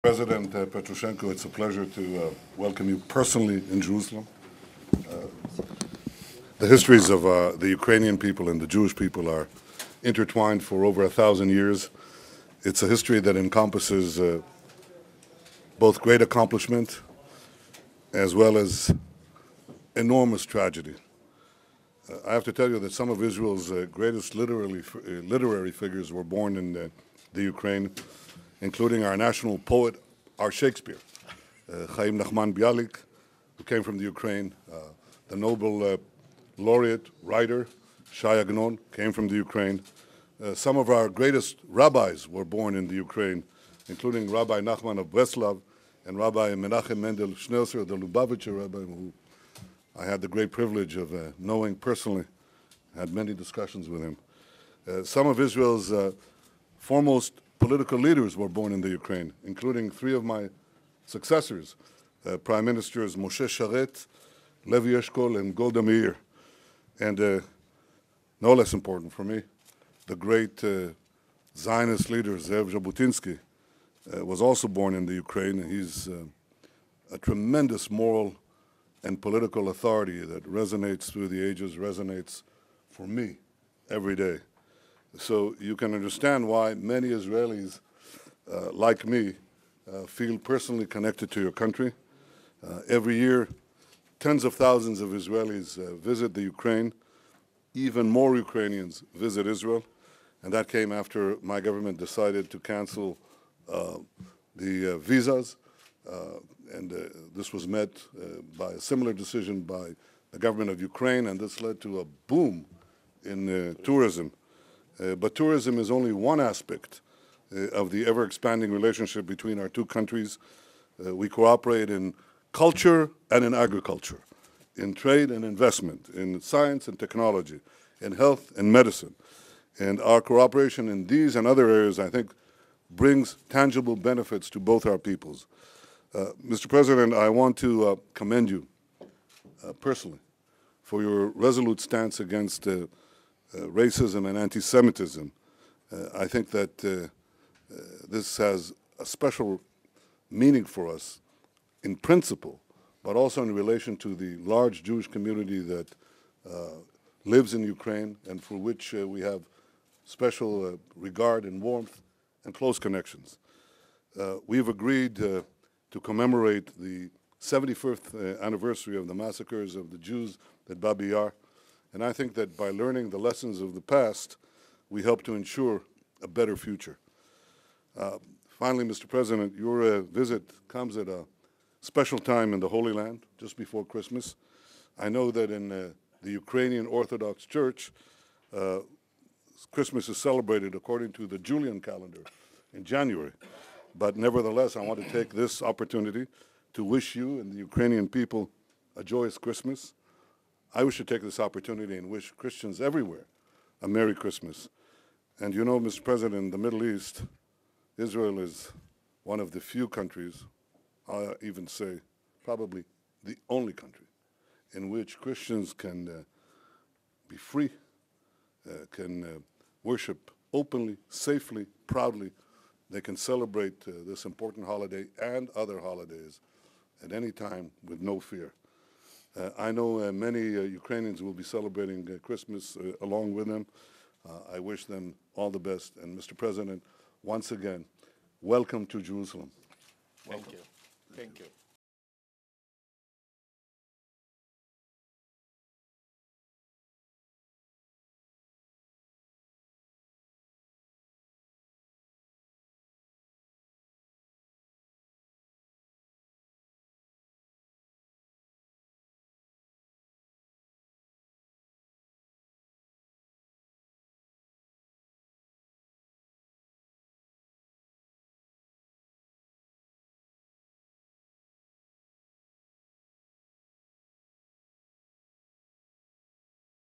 PM Netanyahu Meets with Ukraine President Poroshenko
פגישת ראש הממשלה בנימין נתניהו ונשיא אוקראינה פטרו פורושנקו.
Prime Minister Benjamin Netanyahu meets with Ukraine President Petro Poroshenko

סאונד: איתמר בוטון, לע"מ